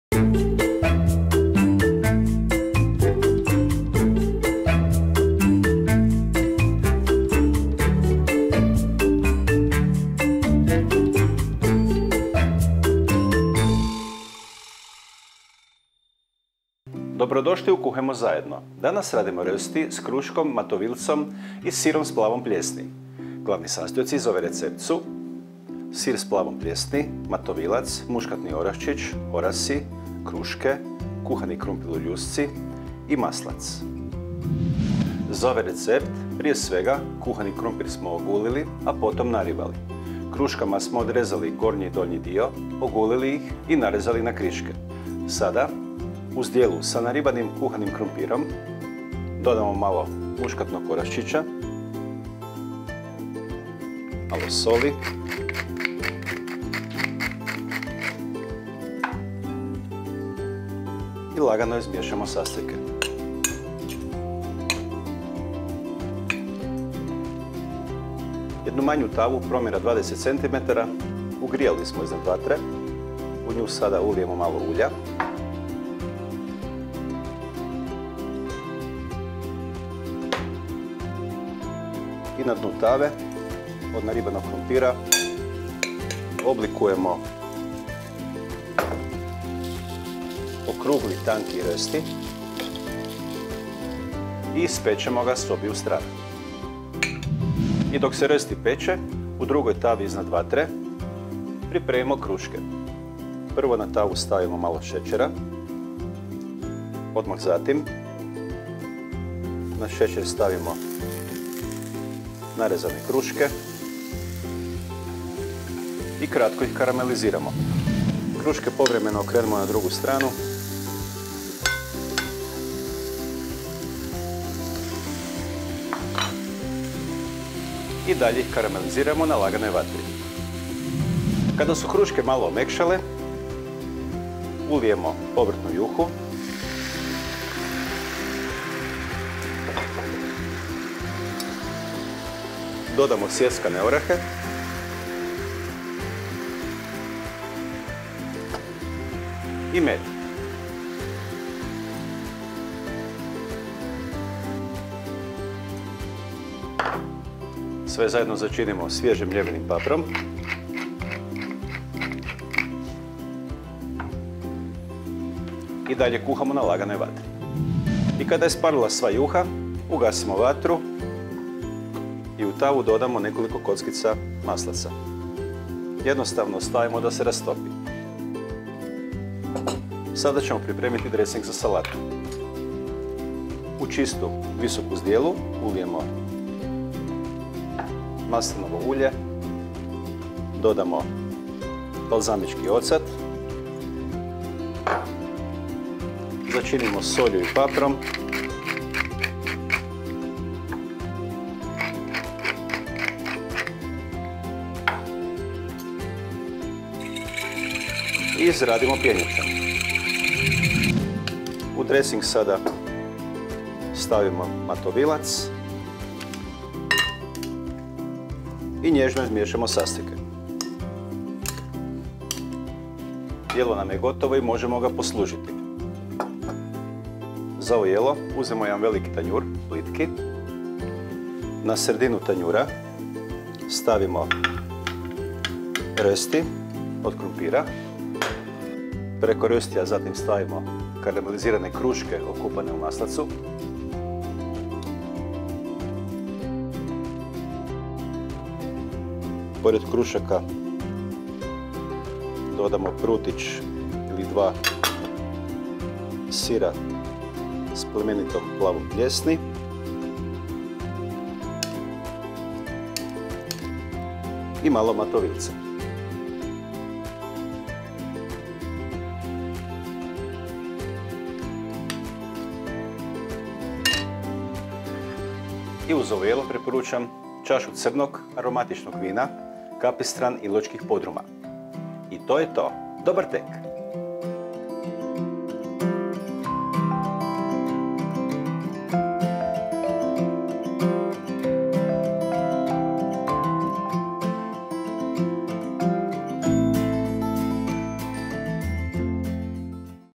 Rosti s kruškom, matovilcem i plemenitim plavim sirom- Fini Recepti by Crochef
Jednostavan recept za pripremu . Porcija je za 4 osobe, a vrijeme pripreme do 20 minuta.

Upute:
Kuhani krumpir ogulimo i naribamo. U zdjelu s krumpirom dodamo muškatnog oraščića, posolimo i izmiješamo sastojke. U zagrijanu malu tavu ulijemo malo ulja i pod dno tave od naribanog krumpira oblikujemo rosti da se peče. Kruške narežemo na kriške, dodamo u veliku vruću tavu zajedno sa šećerom. Kruške okrećemo kako bi se lijepo karamelizirane na laganoj vatri. Kad kruške omekšaju ulijemo povrtnu juhu, dodamo sjeckane orahe i med. Sve zajedno začinim svježe mljevenim paprom. Kad je isparila sva juha u tavu dodamo nekoliko kockica maslaca. U čistu visoku zdjelu napravimo dressing tako da dodamo maslinovo ulje, balzamički ocat, sol, papar i izradimo pjenjačom. U dressing stavimo matovilac i nježno izmiješamo sastojke. Na tanjur stavimo rosti od krumpira, potom slažemo kruške okupane u maslacu, pored krušaka dodamo prutić sira s plemenitom plavom plijesni i mali matovilca.
Sviđa li Vam se ovaj recept? Napišite komentar i podijelite svoje mišljenje!

***Pretplatite se na naš kanal i otkrijte više od 2000 zanimljivih video recepata!***